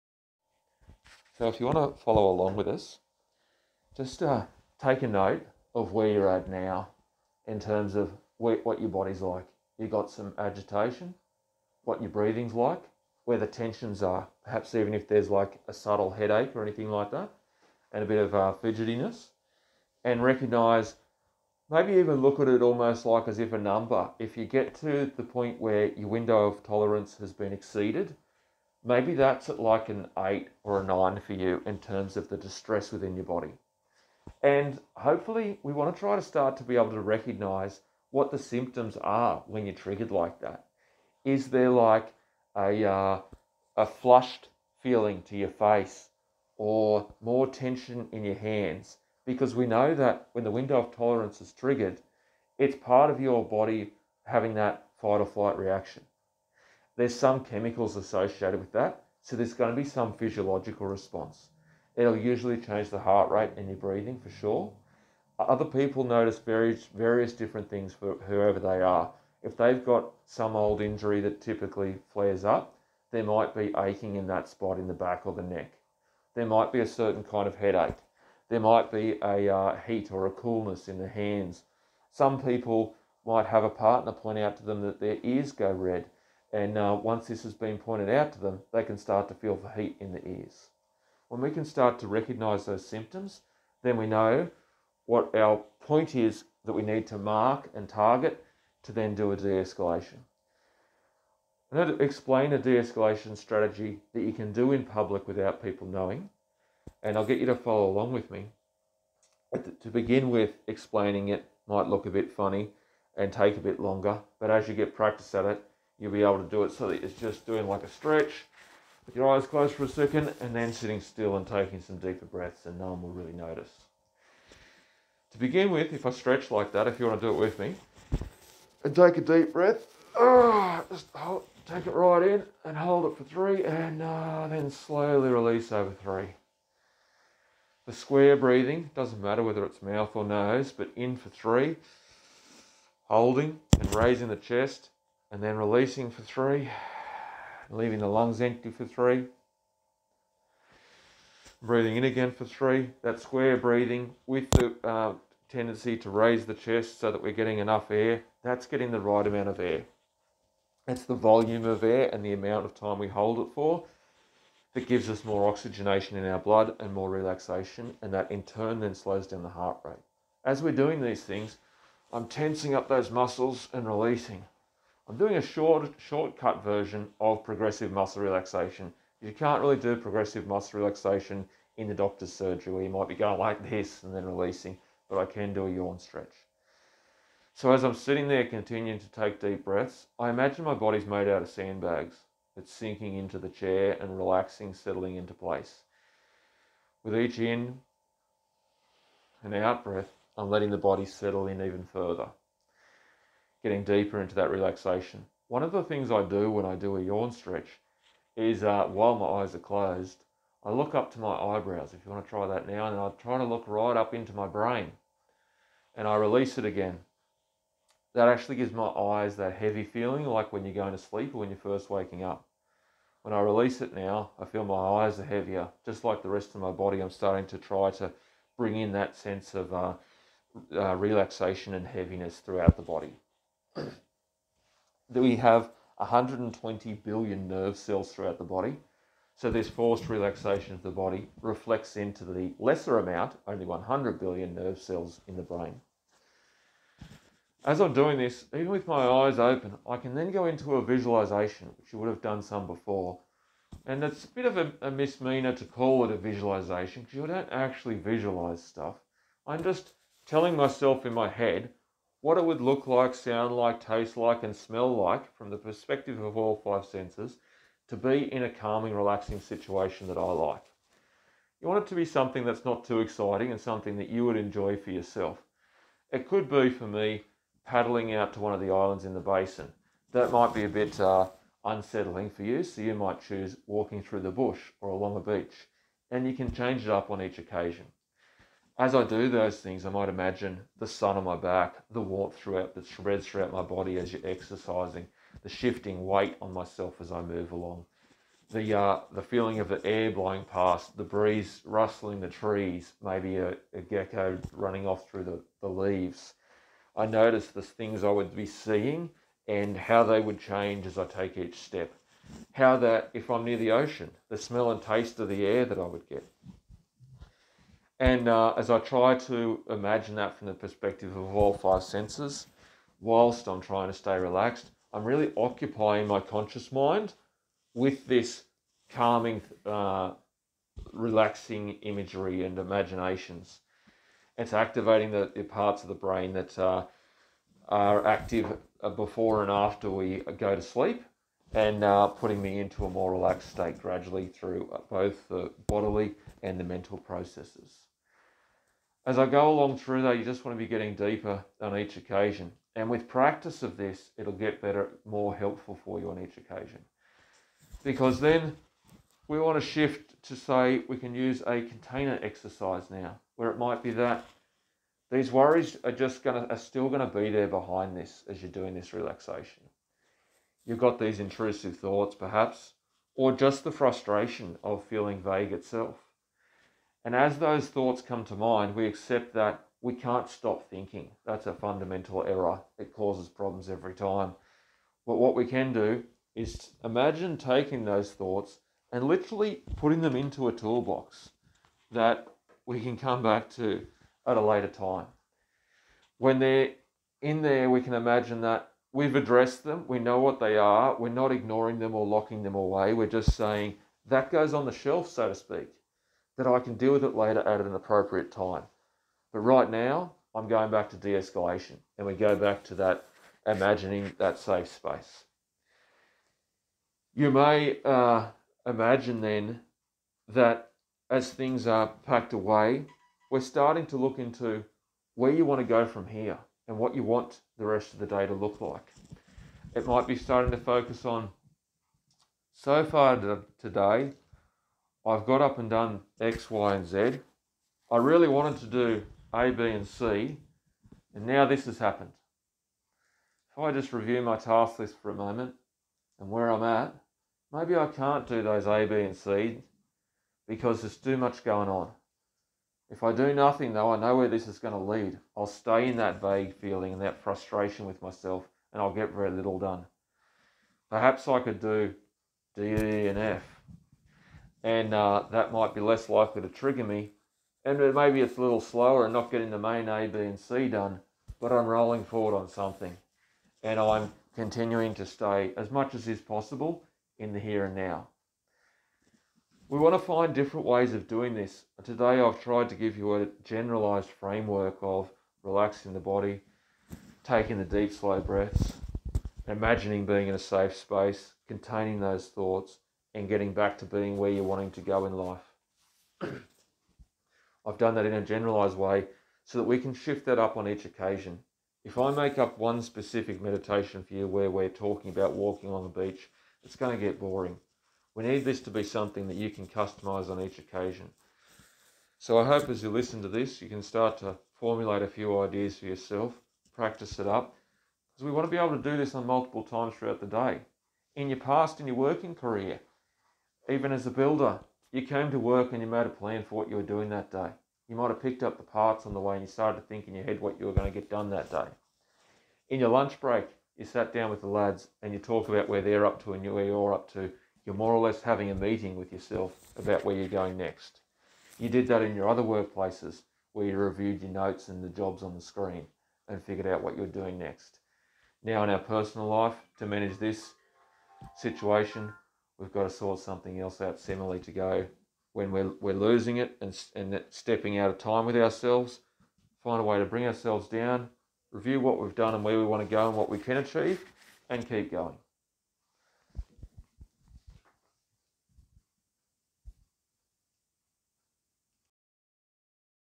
De-escalation and containment instructions for professionals
This brief de-escalation and containment meditation is a useful tool for professionals to help regulate their emotions when their "window of tolerance" has been exceeded. De-escalation and containment meditation can be used to help professionals recognize when their emotions are becoming overwhelming and take steps to regulate them. By creating a mental container for intense emotions and focusing on a positive resource, they can gain more control over their emotional state and maintain a productive and focused mindset.